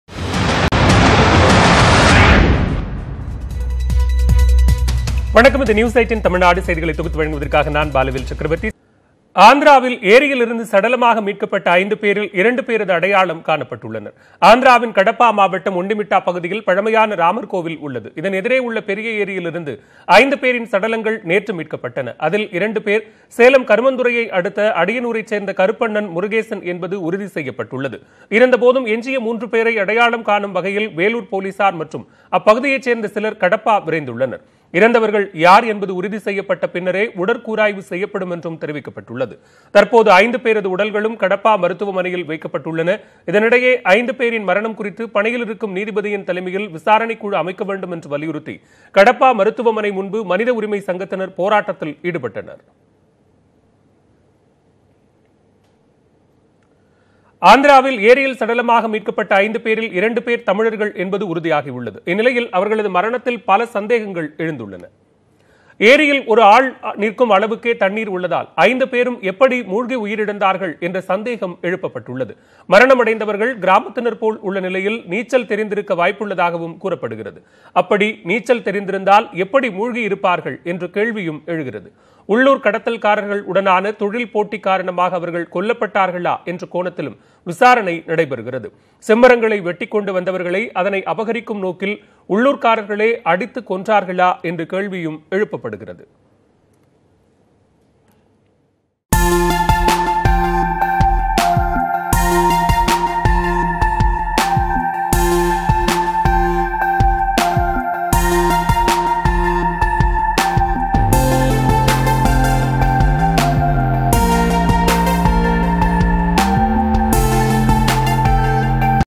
ஆந்திரா ஏரியில் மிதந்த சடலங்களில்,இருவர் அடையாளம் காணப்பட்டது;இதனால் எழுப்பப்படும் சந்தேகங்கள்...
ஆந்திரா ஏரியில் மிதந்த சடலங்களில்,இருவர் அடையாளம் காணப்பட்டது.. ஒருவர் சேலம் மாவட்டம் கருமாந்துறையை சேர்ந்த கருப்பண்ணன் மற்றும் முருகேசன் என தெரிவிக்கப்பட்டுள்ளது.